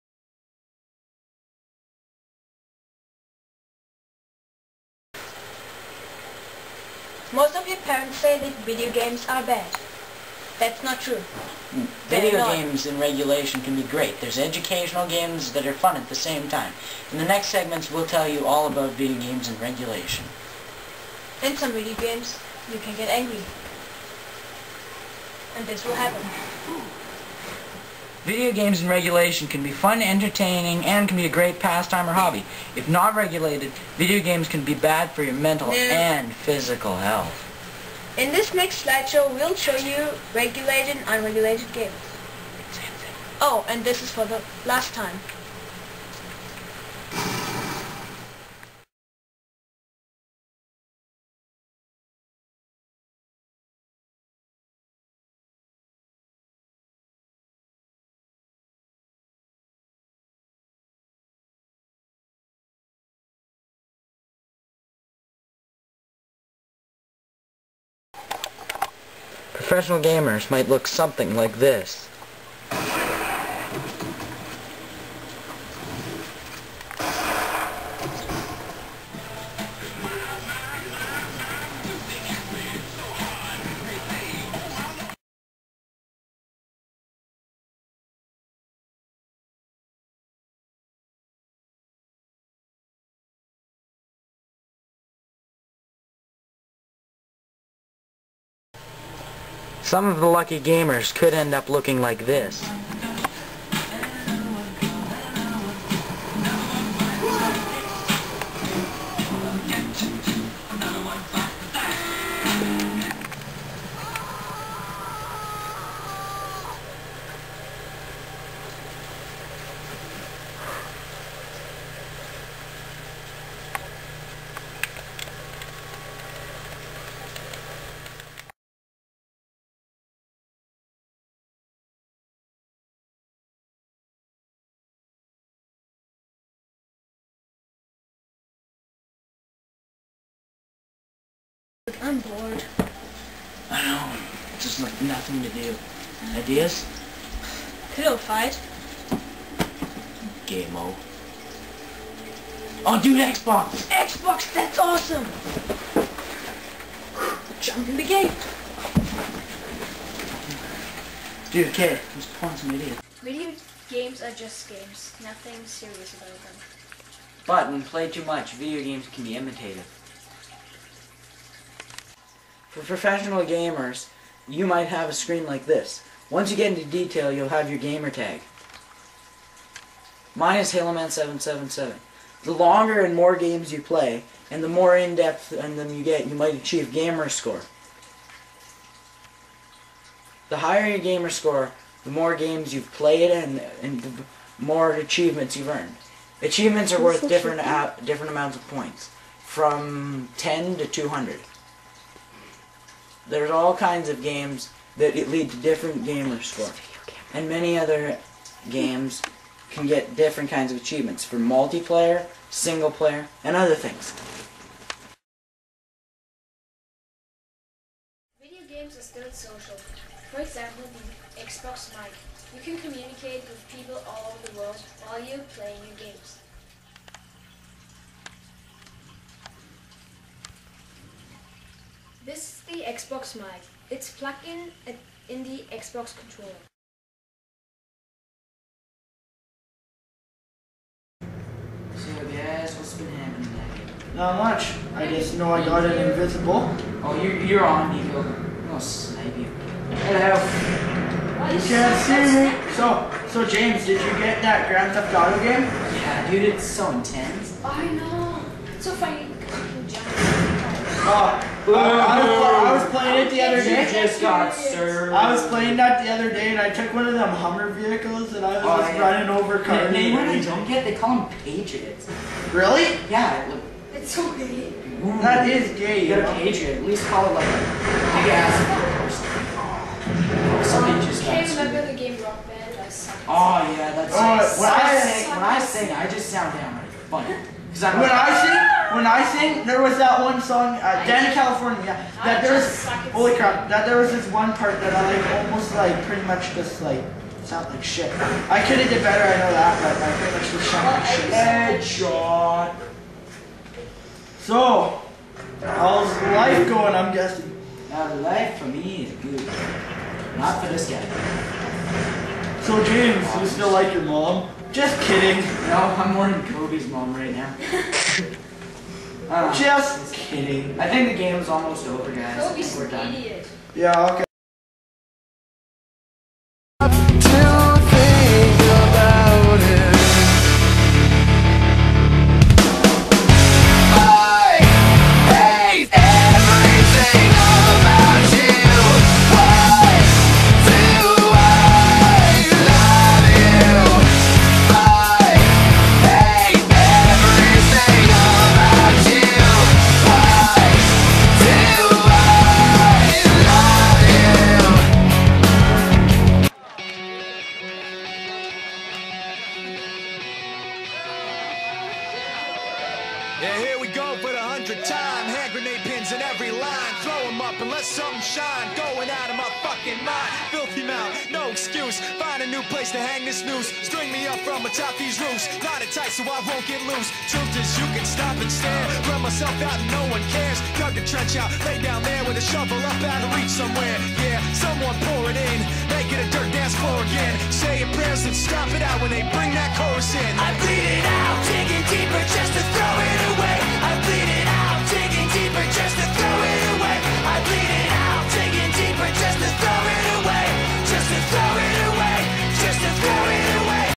Video games in regulation
This is a school project about video games